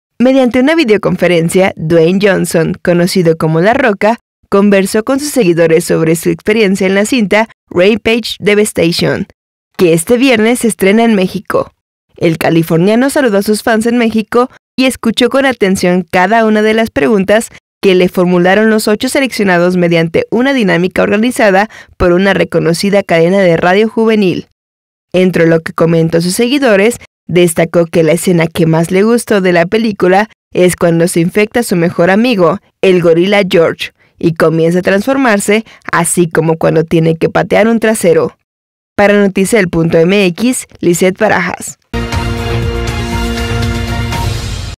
Dwayne Johnson llega con “Rampage Devastacion”
La cinta de acción se estrena el 13 de abril en México.